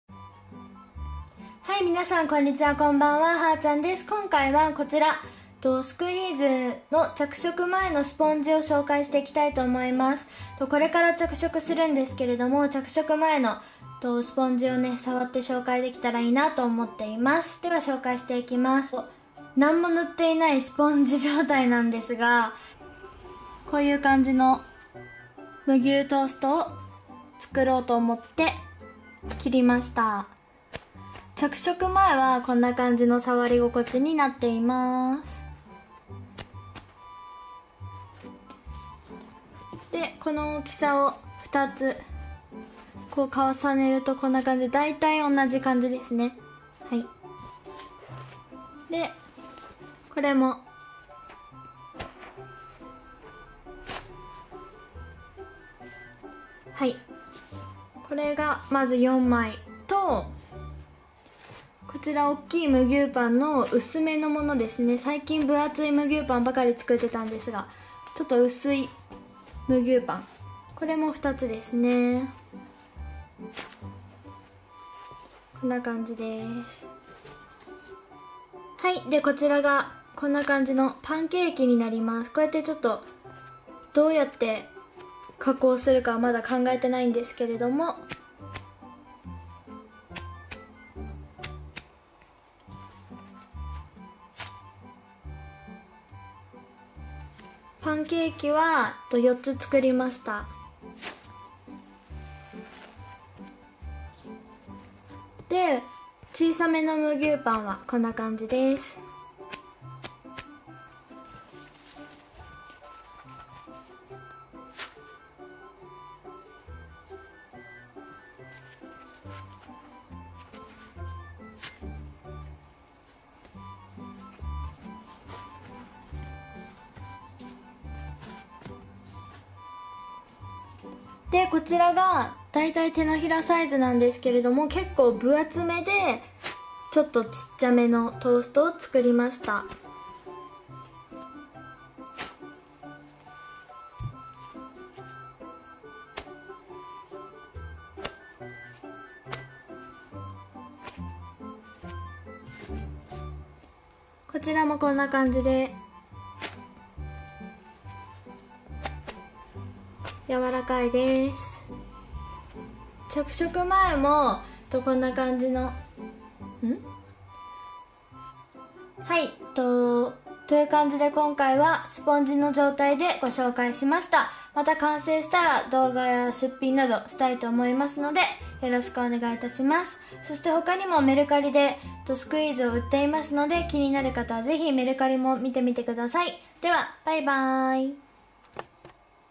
[手作りスクイーズ]着色前のスポンジの触り心地動画
プレゼント企画第２段の商品です！！第１段はもう販売中です！！前回のライブ動画でお知らせしております！